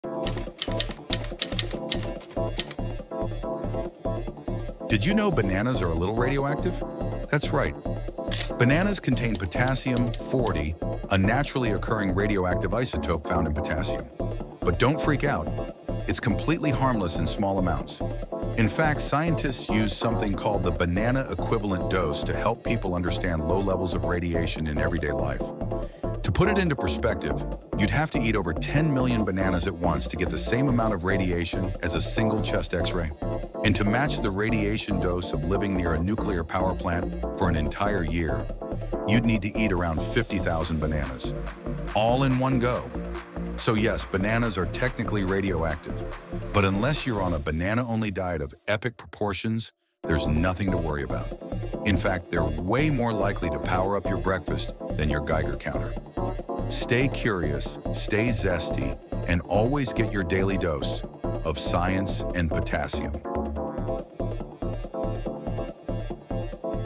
How Radioactive Is a Banana?
Did you know that bananas are slightly radioactive? 🍌😱 It’s true — thanks to a natural element called potassium-40, bananas give off a tiny bit of radiation. But don’t worry, you’d have to eat millions to feel any effects! In this episode, Mr. C explains how bananas compare to a nuclear power plant and why it’s perfectly safe to keep snacking. Science can be weird… and delicious! 😄⚛️